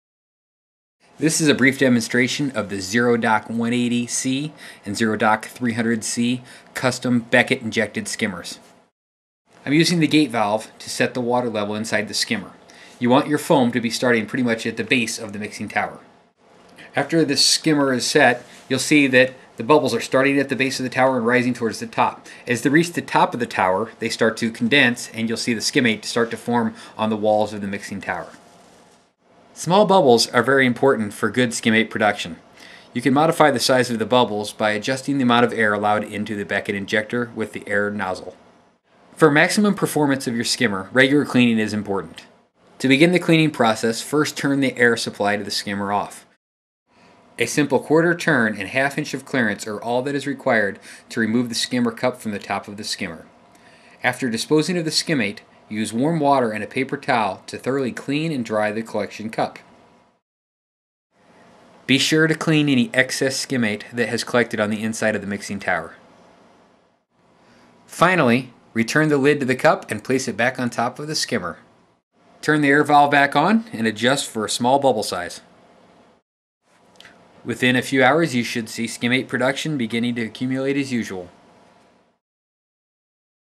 ZeroDOC Protein Skimmer Instructional Video
This is the instructional video for the ZeroDOC (Dissolved organic content) Beckett Injected Protein skimmers.  These are high quality protein skimmers with heat formed boxes that result in 3 less glued seams, compact height and 1.5" gate valves.